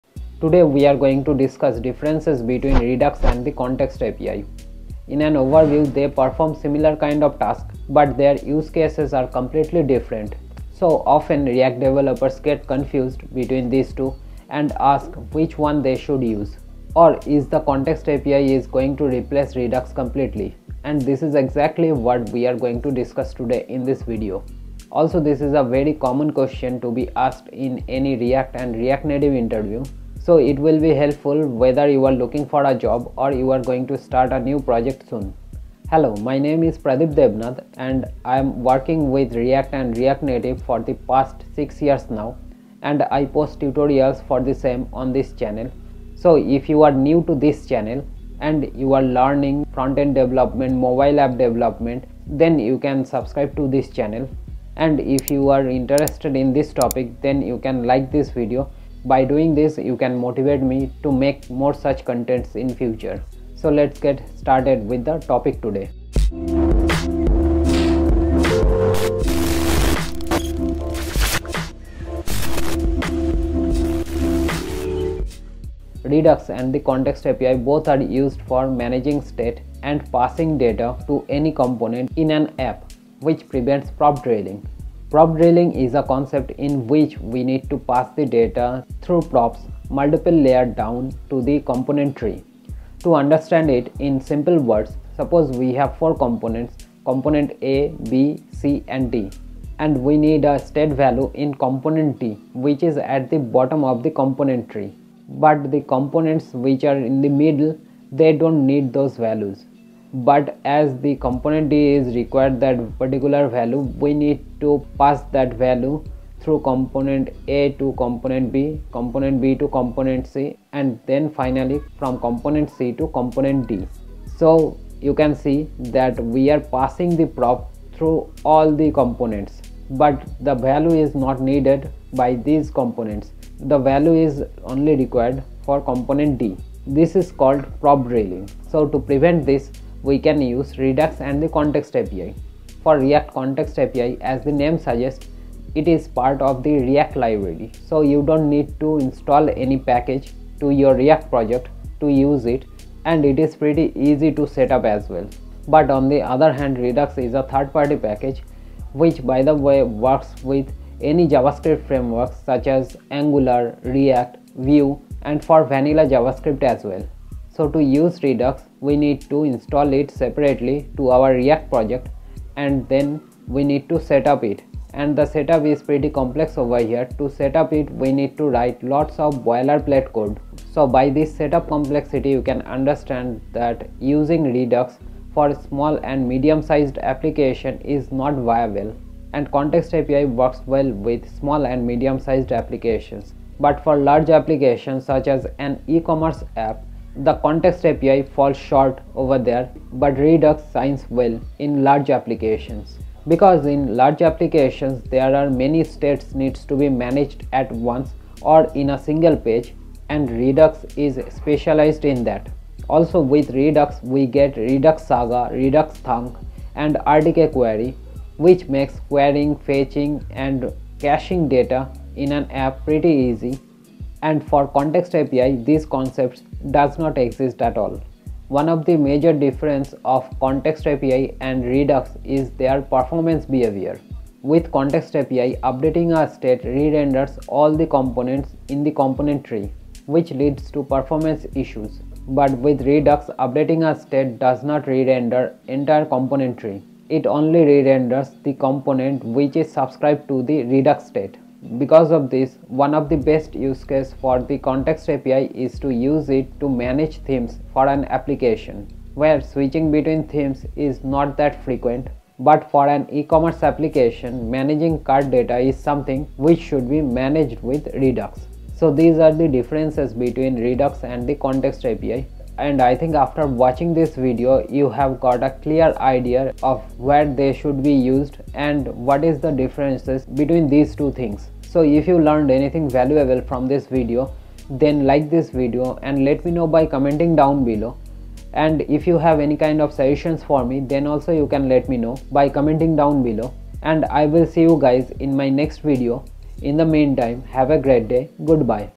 Redux vs React Context API | Which One To Use? React Interview Question 2025
You'll learn the differences between Redux and Context API in this video. If you're confused about which one you should use, then this video is the answer to it. Also, this is a commonly asked question in any react or react native interview. So if you're preparing for a front-end React developer job or React native mobile application developer then this tutorial is one stop solution for you.

For more tutorials on WordPress, React JS, React Native and Flutter